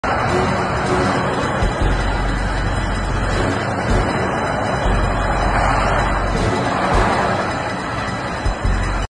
oil and gas platform is on fire in the Gulf of Mexico
The Pemex oil and gas platform is on fire in the Gulf of Mexico. As a result of the explosion, two workers were killed, 8 more were injured. One person went missing.